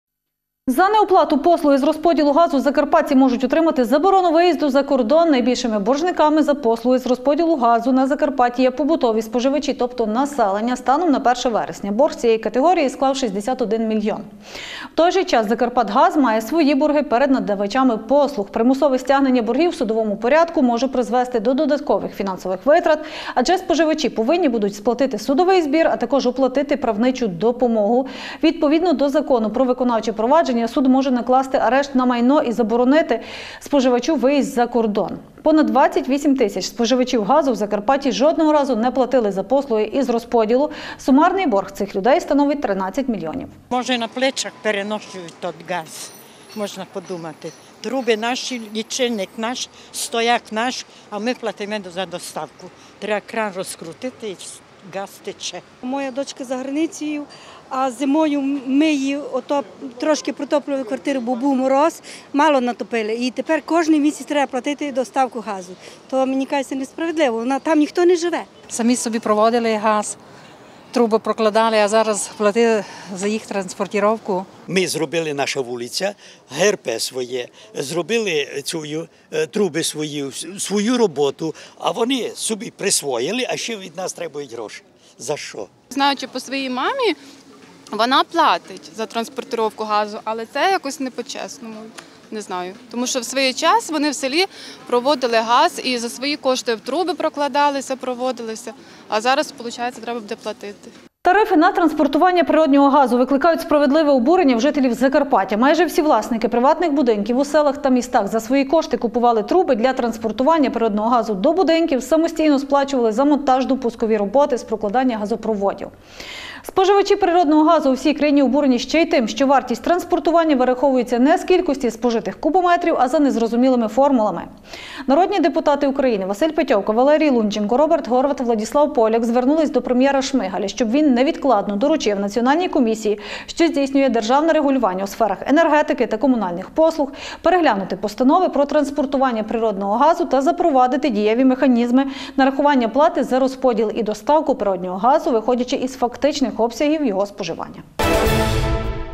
Переглянути тарифи на транспортування газу. Жителі Закарпаття обурені оплатами
Закарпатці кажуть: їх обурюють рахунки за транспортування газу, які вони вважають несправедливими. Свого часу люди самі проводиои мережі до будинків, а зараз мають ще й платити за це . Більше того - оплату просять і за транспортування газу в тих будинках, які ще не збудовані. Наародні депутати Василь Петьовка, Валерій Лунченко, Роберт Горват, Владіслав Поляк звернулись до Прем'єра терміново переглянути скандальні тарифи